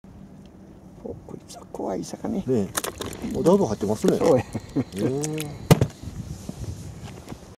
アイゴ（バリコ）の途中釣果
２０センチほどのアイゴ（バリコ）ですが、面白いようにウキが入っていきます。さて、この後はどれくらい釣れたのでしょうか？
４月１２日、みなべ堺堤防。